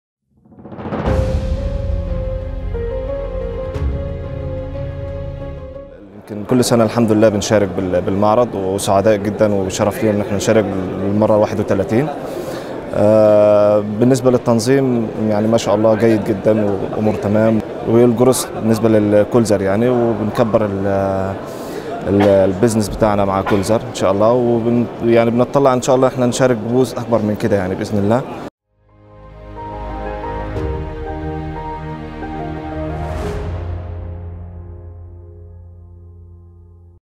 مقابلة أحمد حموده، مندوب مبيعات الحمادي للخدمات الطبية خلال المؤتمر السعودي العالمي لطب الأسنان 2020
Interview with Ahmed Hamoda – Sales Representative at Al Hammadi Medical Services – during SIDC 2020